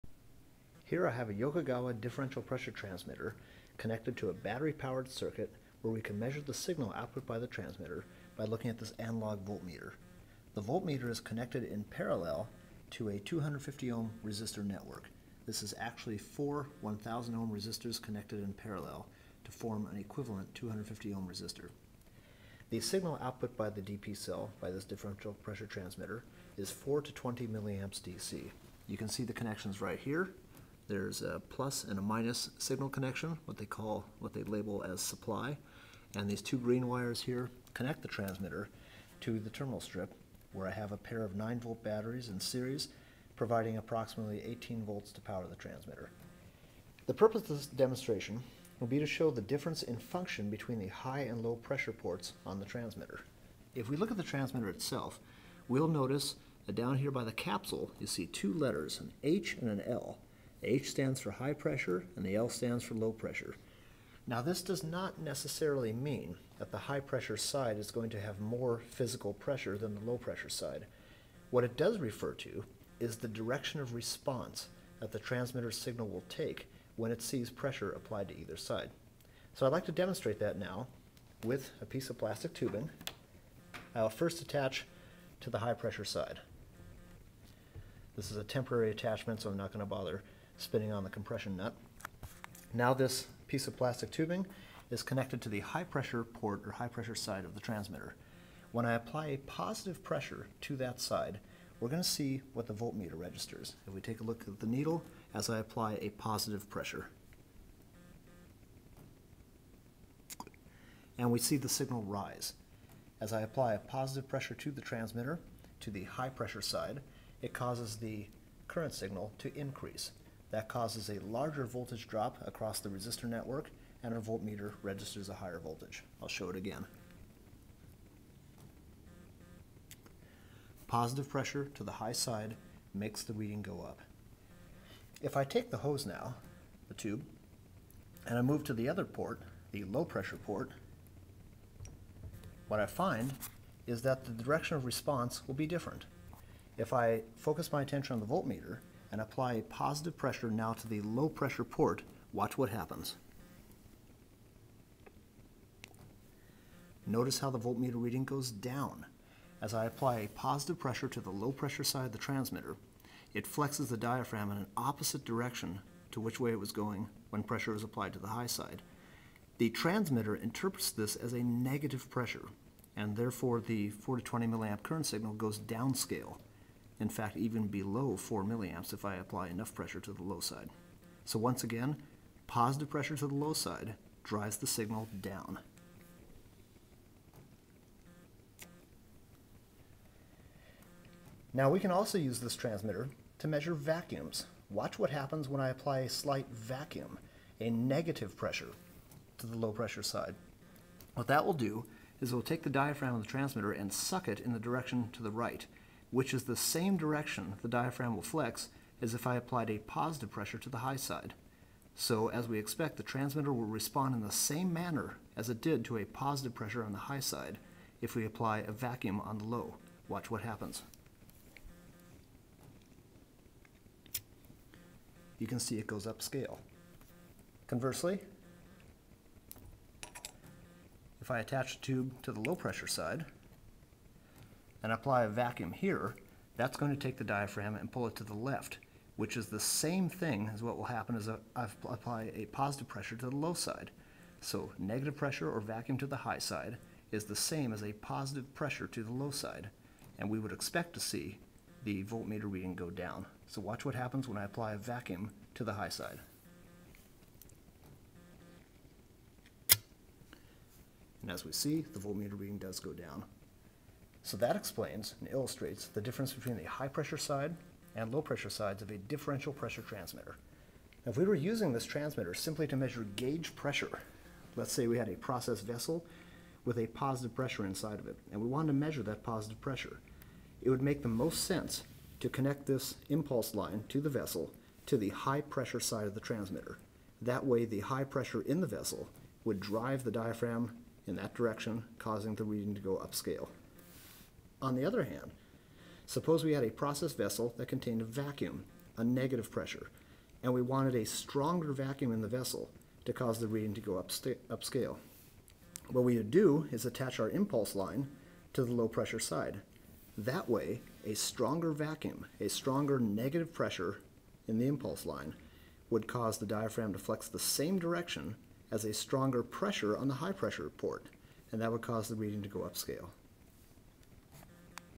Differential pressure transmitter response -- "high" and "low" ports
This video demonstrates the different responses of a "dp cell" differential pressure transmitter to both positive and negative pressures applied to its ports.  The response of pressure and vacuum to the "high" port is opposite the effect of pressure and vacuum (respectively) applied to the "low" port.